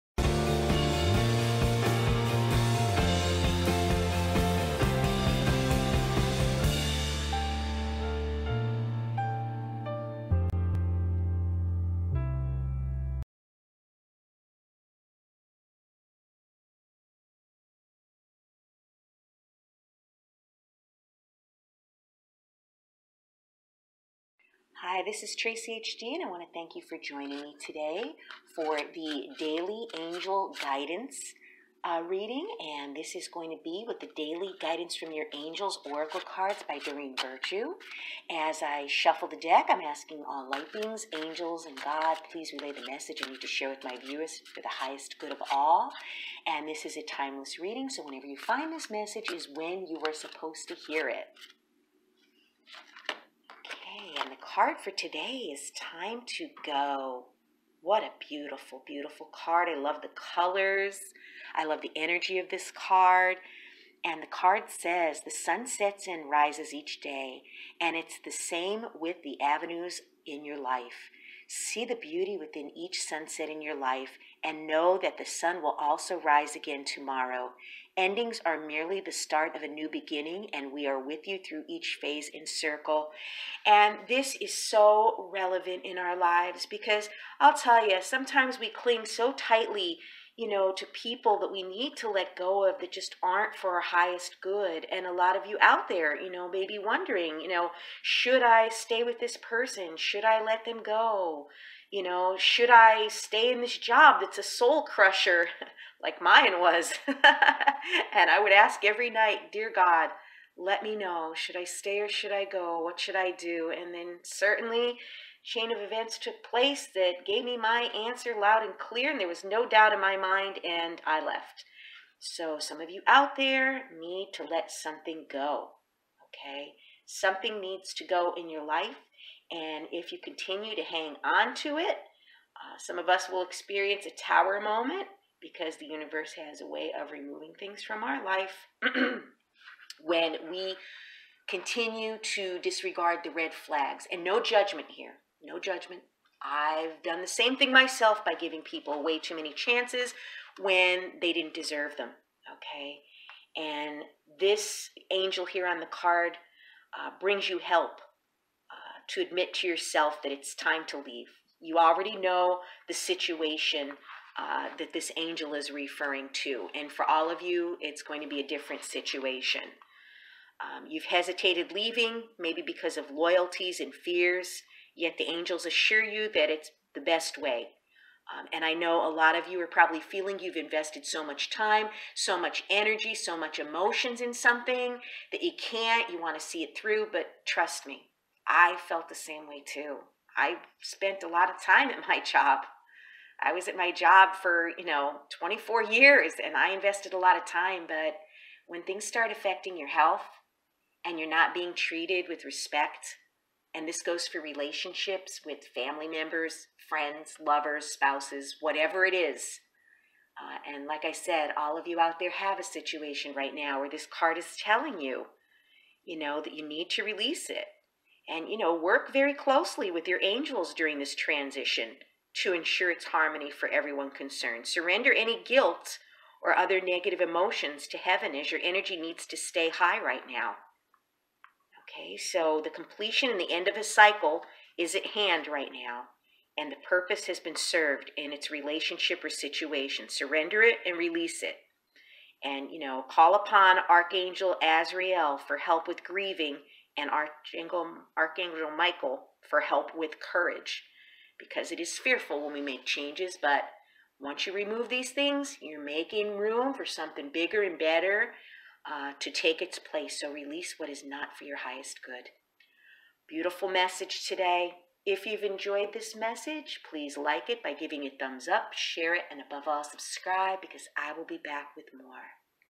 THE DAILY ANGEL GUIDANCE MESSAGE
Traceyhd gives the daily angel guidance message. This reading is timeless and meant to be heard when you find this video.  What is the message the angels want you to know. Watch and find out. Don't miss a reading.